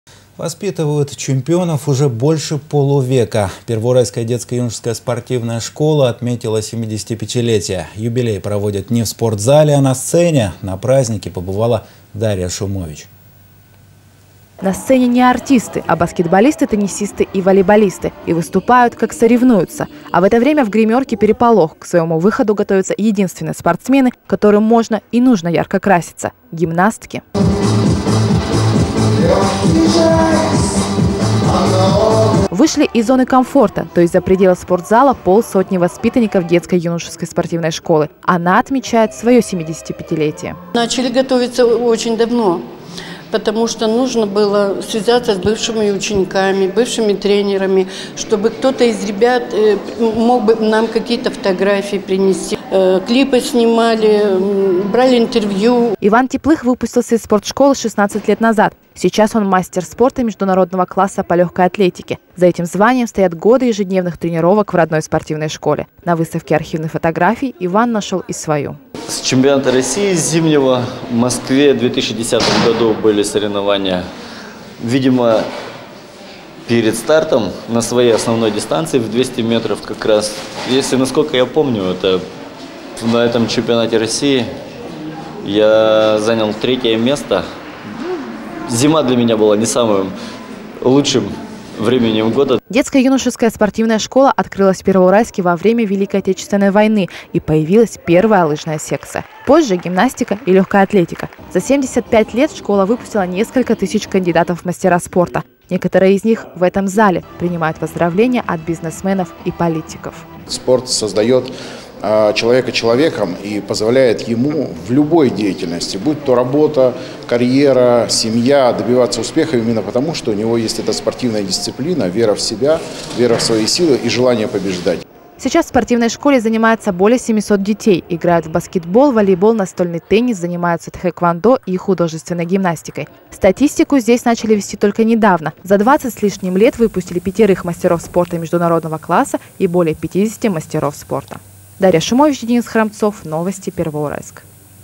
Воспитывают чемпионов уже больше полувека
Первоуральская детско-юношеская спортивная школа отметила 75-летие. Юбилей проводят не в спортзале, а на сцене. На празднике побывала Дарья Шумович.

На сцене не артисты, а баскетболисты, теннисисты и волейболисты. И выступают как соревнуются. А в это время в гримерке переполох. К своему выходу готовятся единственные спортсмены, которым можно и нужно ярко краситься - гимнастки.

Вышли из зоны комфорта, то есть за пределы спортзала, полсотни воспитанников Детско-юношеской спортивной школы. Она отмечает своё семидесяти пятилетие.

Валентина ИВАНОВА, директор ДЮСШ: «Начали готовиться очень давно, потому что надо было связаться с бывшими учениками, бывшими тренерами, что бы кто-то из ребят смог фотографии нам принести, клипы  снимали, брали интервью».

Иван Теплых выпустился из спортшколы 16 лет назад. Сейчас он мастер спорта Международного класса по легкой атлетике. За этим званием стоят годы ежедневных тренировок в родной спортивной школе. На выставке архивных фотографий Иван нашел и свою.

Иван ТЕПЛЫХ, выпускник ДЮСШ: «С Чемпионата России зимнего, в Москве в 2010  году  были соревнования. Видимо, перед стартом на своей основной  дистанции в 200 метров как раз, насколько я помню, на этом Чемпионате России я занят третье место, зима для меня была не  самым лучшим временем года».

Детско-юношеская спортивная школа открылась в Первоуральске во время Великой Отечественной войны. И появилась первая лыжная секция. Позже гимнастика и легкая атлетика. За 75 лет школа выпустила несколько тысячи кандидатов в мастера спорта. Некоторые из них в этом зрительном зале. Принимают поздравления от бизнесменов и политиков.

Александр ХАНИН, начальник управления по взаимодействию с органами власти и общественными организациями ПНТЗ: «Спорт очень дисциплинирует, спорт создает  человека - человеком, позволяет ему в любой деятельности будь то работа, карьера, семья, добиваться успеха именно потому что у  него есть эта спортивная  дисциплина, вера в себя, вера в свои силы и желание побеждать».

Сейчас в спортивной школе занимается более 700 детей. Играют в баскетбол, волейбол, настольный теннис, занимаются тхэквандо и художественной гимнастикой. Статистику здесь начали вести только недавно. За двадцать с лишним лет выпустили пятерых мастеров спорта международного класса и более 50 мастеров спорта.